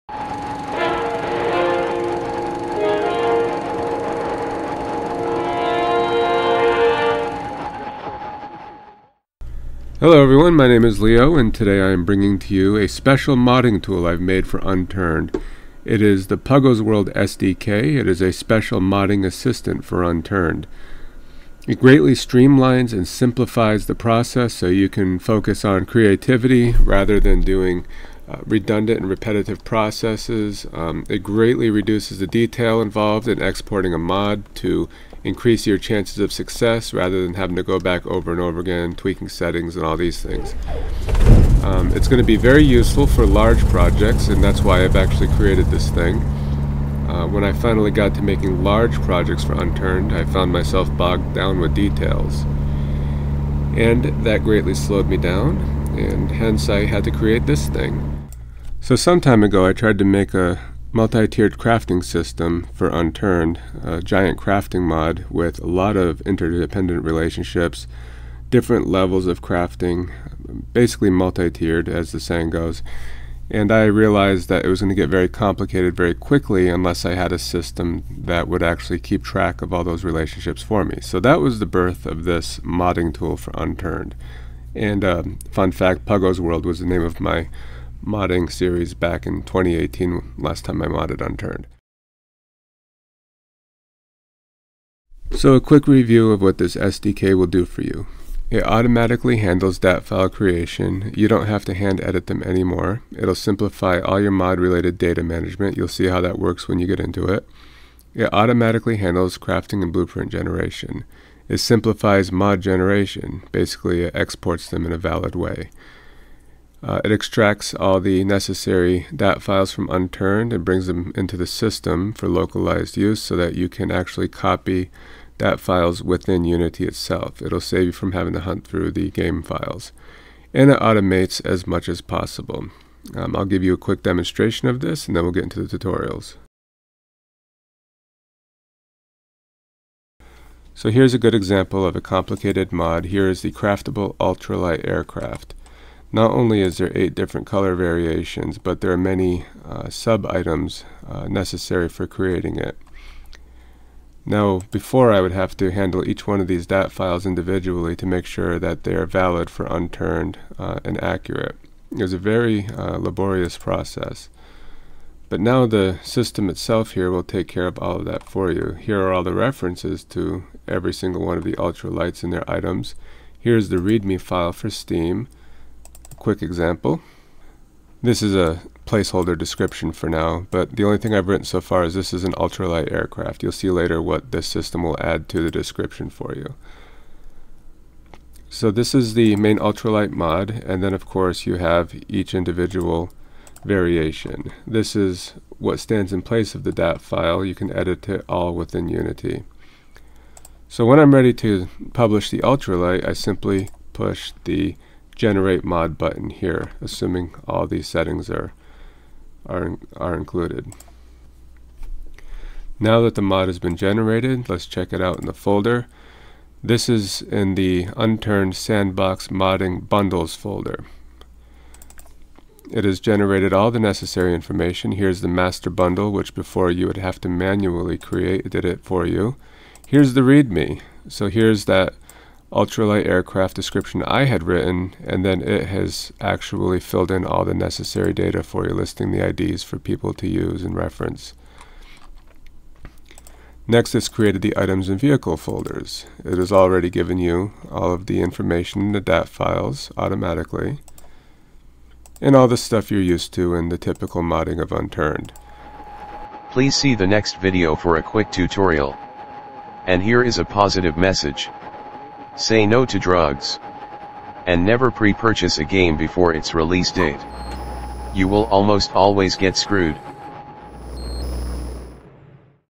Tutorial #1: Introduction. PuggosWorldSDK - Unturned Modding Tool.
This is a modding tool for the Game Unturned. See documentation